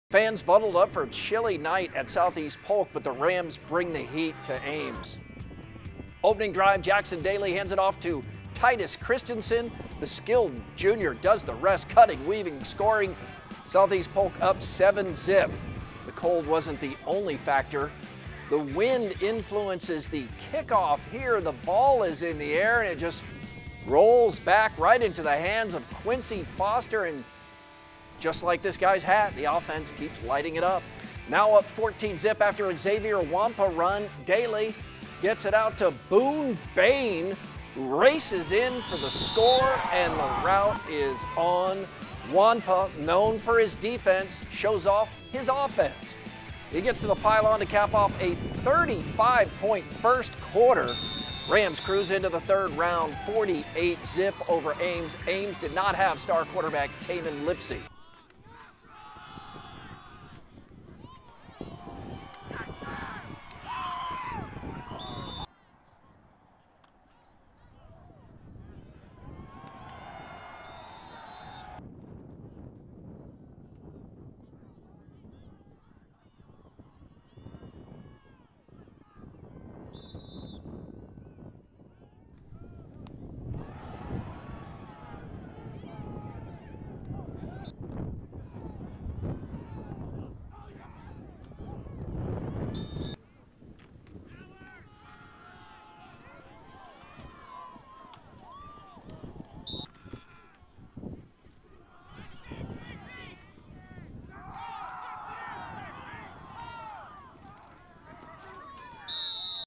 Football Highlights: Ames at Southeast Polk 10-23-20
It was an Iowa High School Football second round 4A playoff showdown as the Ames Little Cyclones (2-4) traveled to take on the Southeast Polk Rams (6-0) on October 23rd.
On paper, it seemed like a tough matchup for the Little Cyclones and that was also the reality as the Rams proved why they are the undefeated, No. 1 team in the state with a dominant 48-0 victory. Southeast Polk scored 35 of those points in the first quarter and racked up 330 yards on the ground, while only yielding four (43 total yards) defensively. Check out the dominant highlights that begin with what was broadcast on WHO 13, followed by the rest of what I put together!